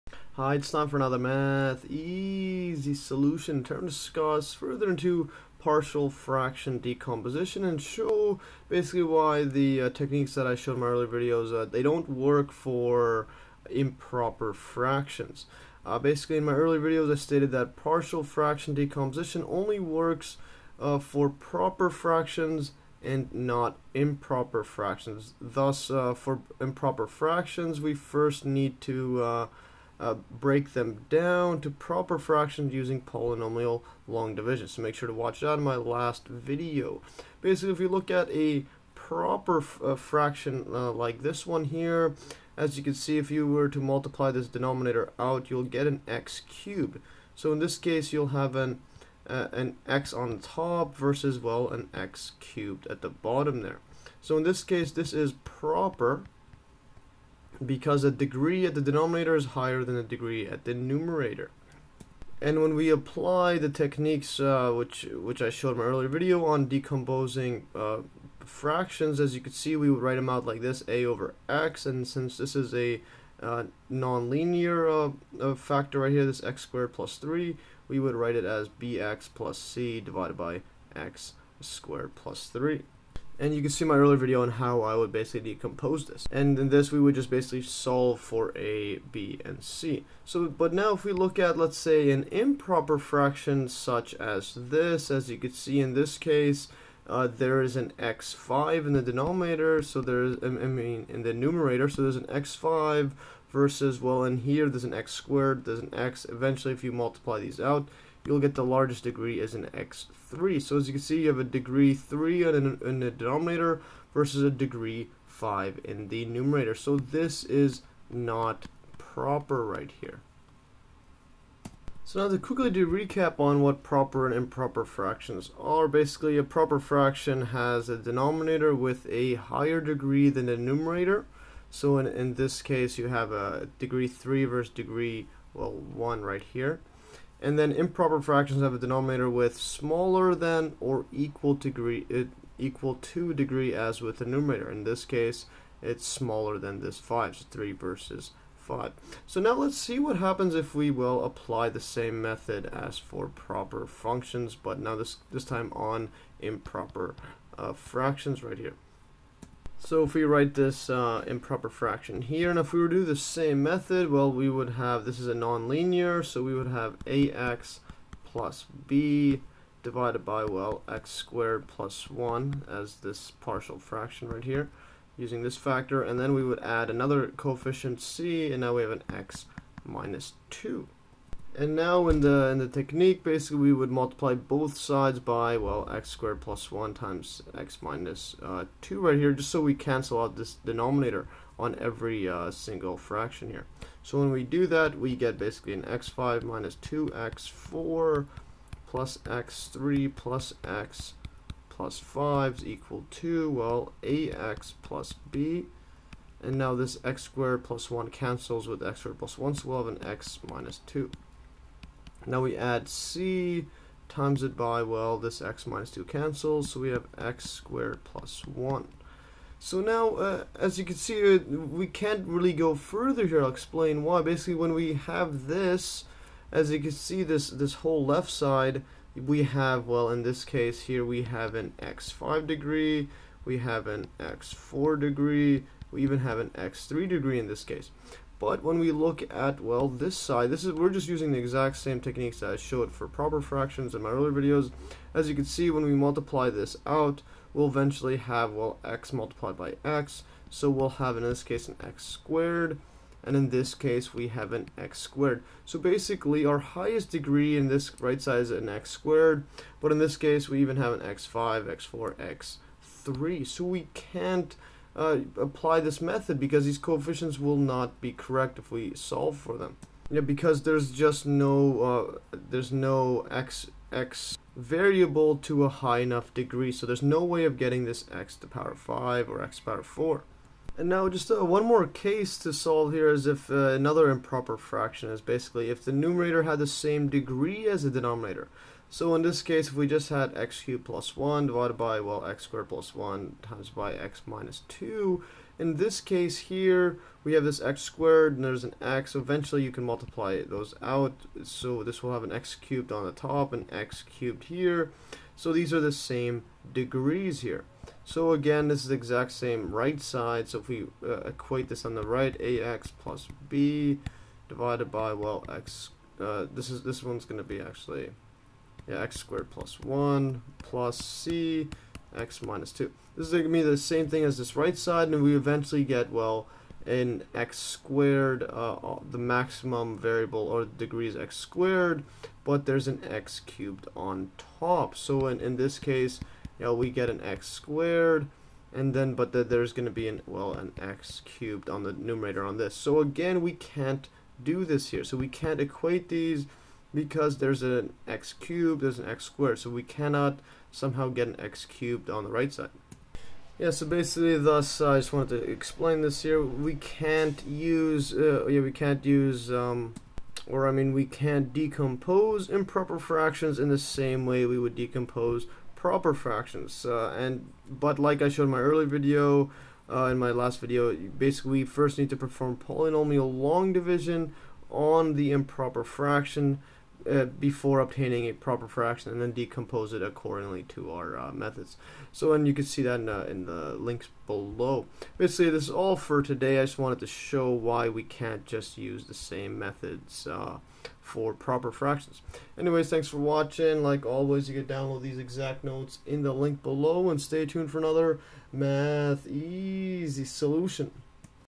Partial Fraction Decomposition: Why it doesn't for Improper Fractions
In this video I go over further into partial fraction decomposition and this time explain why the techniques shown in my earlier videos for decomposing proper fractions don't work for improper fractions. Thus to decompose improper fractions we first need to perform polynomial long division to obtain proper fractions.